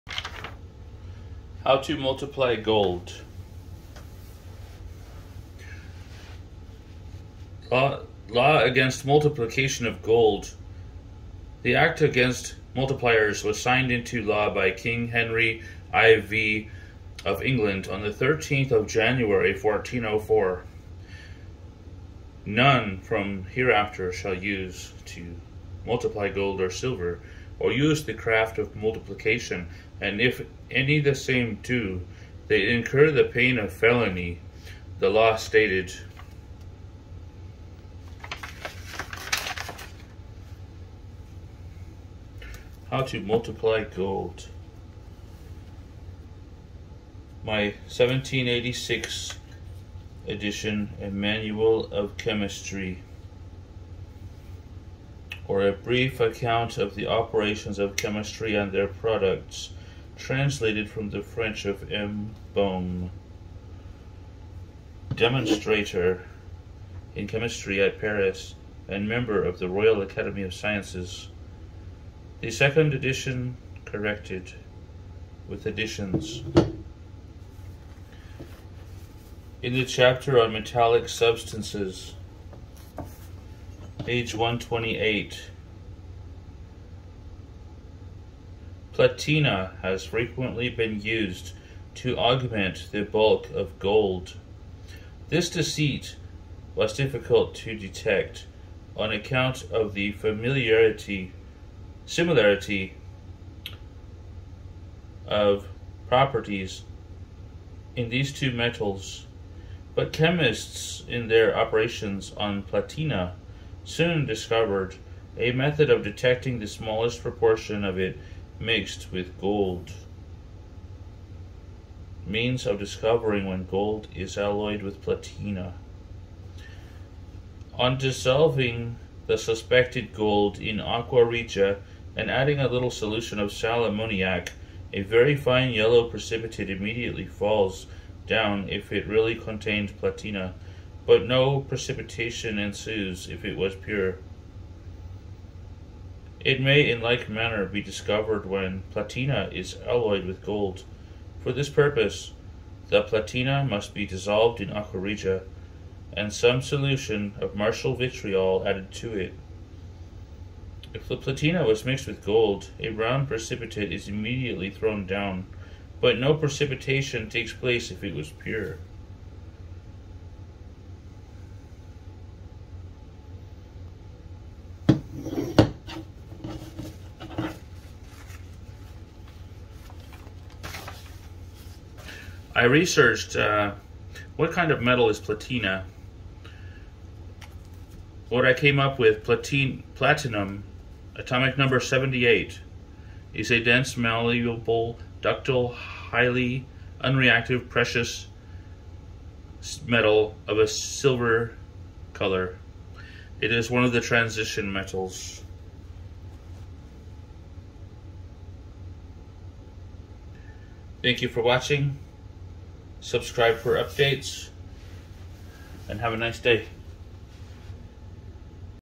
How To Multiply Gold Rare Alchemy Science Book
Law against multiplication of gold.
The act against multipliers was signed into law by king Henry IV of England on the 13th day of January 1404. "None from hereafter shall use to multiply gold or silver, or use the craft of multiplication; and if any the same do, they incur the pain of felony".

A Manual of Chemistry (Modern Reprint Edition). From my 1786 edition page 128 under the chapter on metallic substances;

"Platina has frequently been used to augment the bulk of gold. This deceit was difficult to detect, on account of the similarity of properties in these two metals. But chemists, in their operations on platina, soon discovered a method of detecting the smallest proportion of it mixed with gold.

In modern times I have heard of persons doing multiplication of gold by alloying it with copper through melting the two together. These instances were often blamed on alchemy as well as alchemists in general, however these types of deceit were not associated with the Philosopher's stone of alchemy, nor were they part of alchemical transmutations of metals. Two completely different areas of study.

My research on what kind of metal is platina? revealed platinum, atomic number 78 is a dense white silver colored transition metal that is malleable, ductile, and highly unreactive. In modern times, platinum is too expensive to multiply gold, and this is probably why charlatans switched to using copper, falsely giving alchemists a bad name.